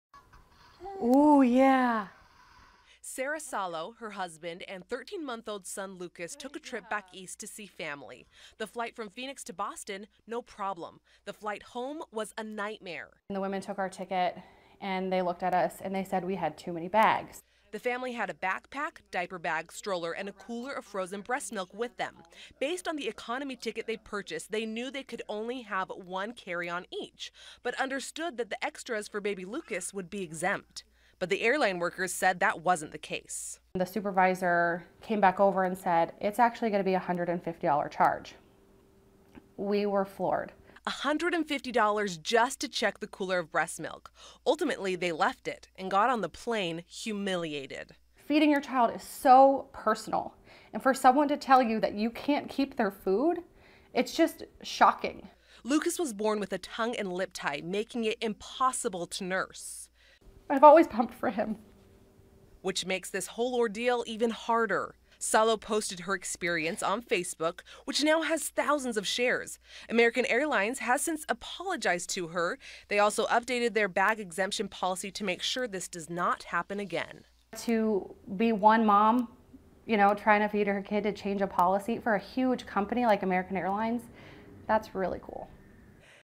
VIDEO: Mesa mom says she was shamed for trying to bring breast milk on plane
A Mesa mom's social media post about her battle with American Airlines has gone viral. She says she was shamed&nbsp;for trying to bring breast milk on a plane. (December 15, 2017)